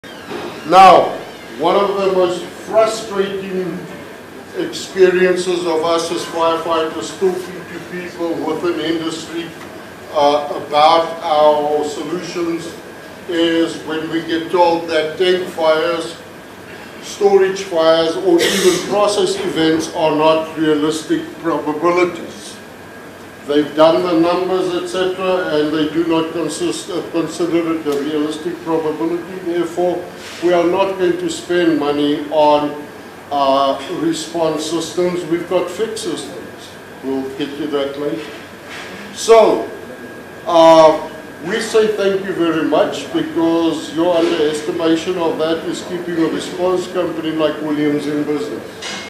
WFHC - Storage tank fire probabilities
Now... one of the most frustrating experiences of us as firefighters or for to people within the industry about our solutions is when we get all that tank fires, storage fires or even process events are not realistic probabilities. They've done the numbers etcetera and they do not consist of considering the realistic probability therefore they are not going to spend money on a response systems, we've got fixed systems. We've get you that late. So... we say thank you very much because your underestimation on that is keeping a response company like WILLIAMS in business.